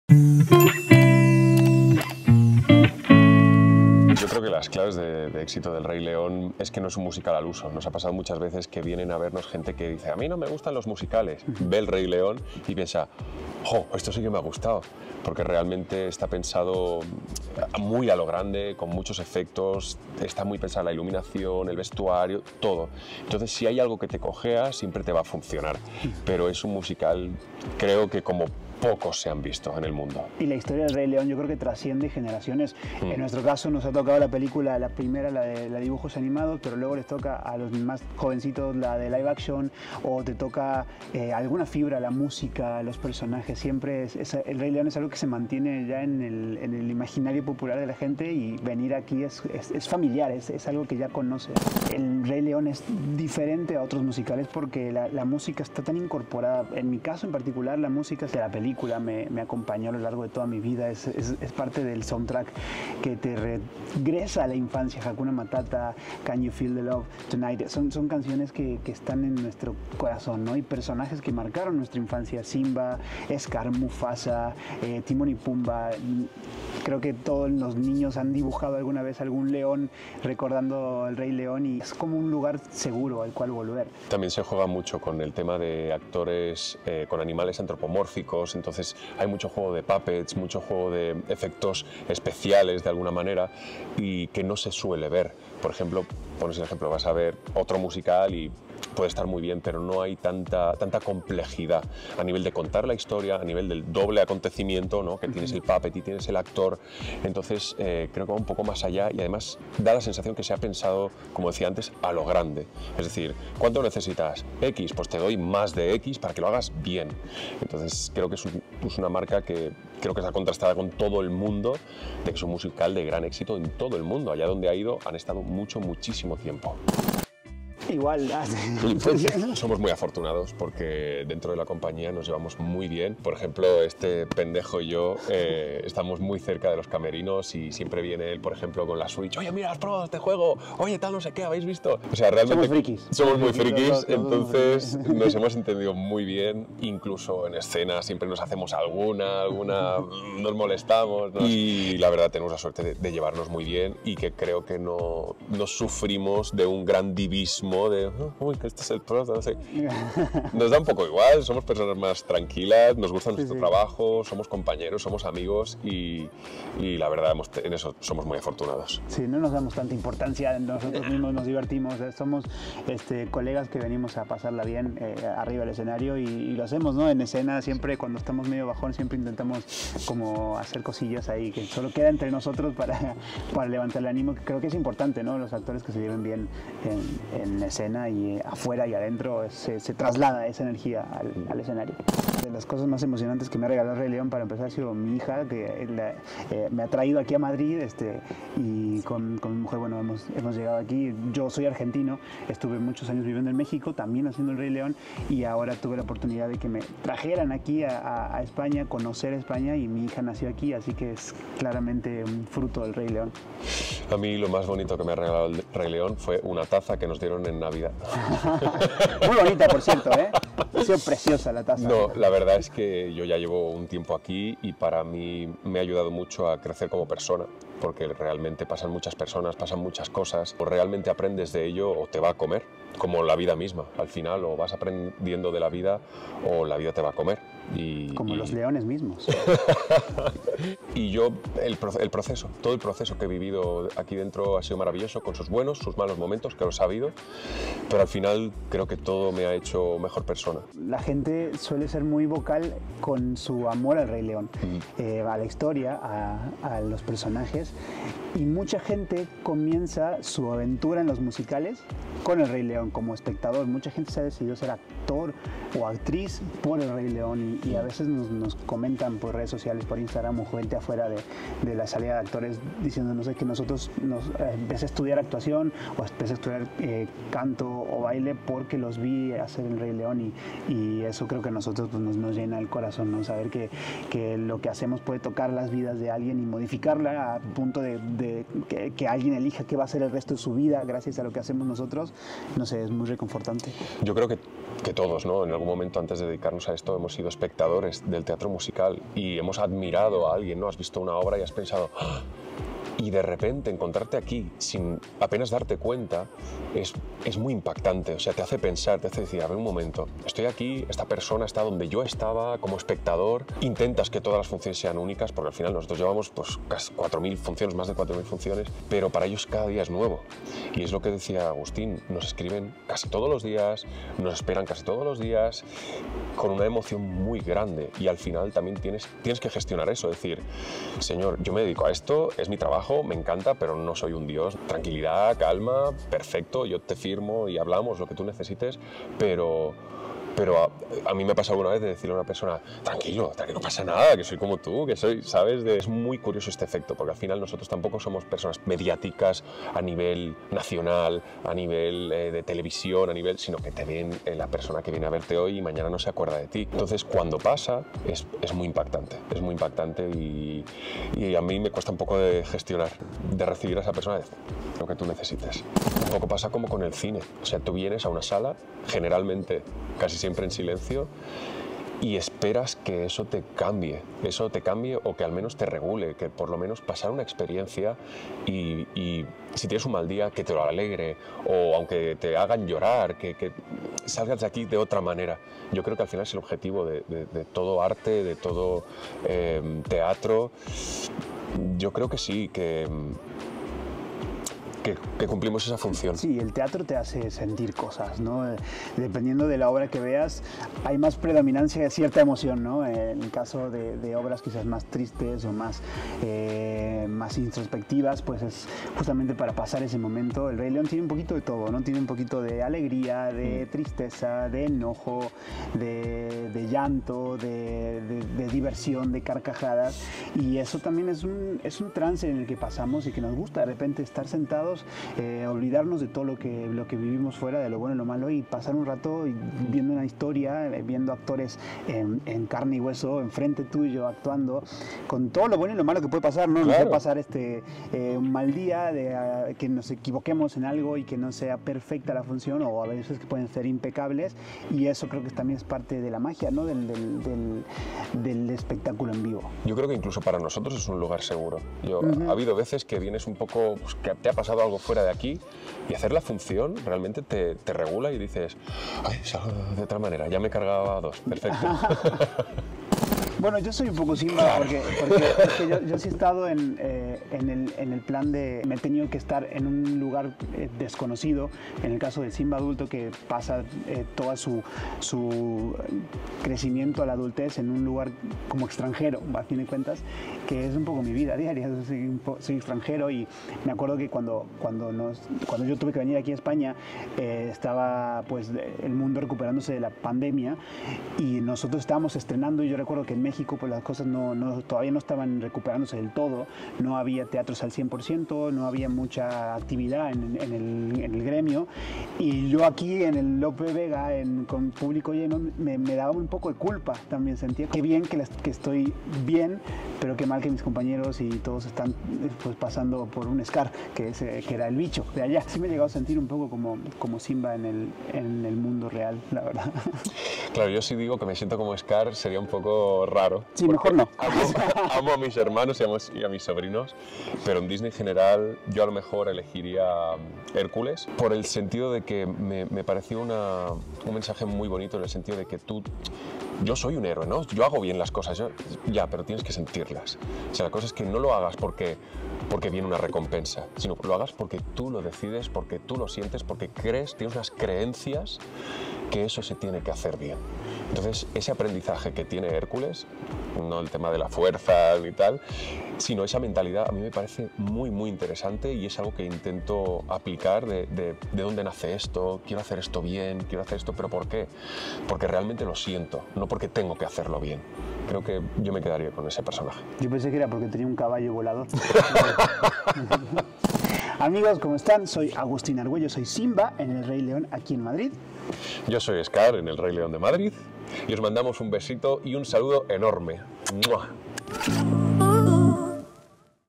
El REY LEÓN cumple 13 años reinando en la Gran Vía de Madrid | ENTREVISTA SHANGAY
| El Teatro Lope de Vega de Madrid vuelve a acoger, para su temporada número trece, al musical más visto de la historia de nuestro país y que ha hecho felices a más de 110 millones de personas. Entramos en las bambalinas de @reyleonmusical para conocer a fondo a @agustinarguello y @pitu_manubens, los actores que encarnan a Simba y Scar.